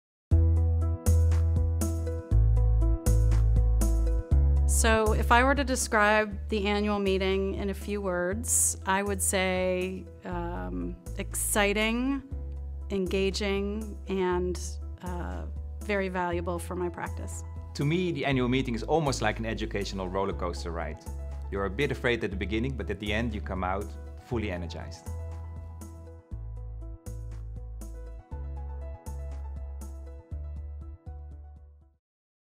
Reasons to Register for INTA's 141st Annual Meeting: Educational Roller Coaster Ride
Ronald van Tuijl (JT International, Geneva, Switzerland) and Cindy Walden (Fish & Richardson, Boston, Massachusetts), Co-Chairs of INTA's 141st Annual Meeting, May 18-22, in Boston, Massachusetts discuss how the Meeting is like an "educational roller coaster ride" that is exciting, engaging, and valuable.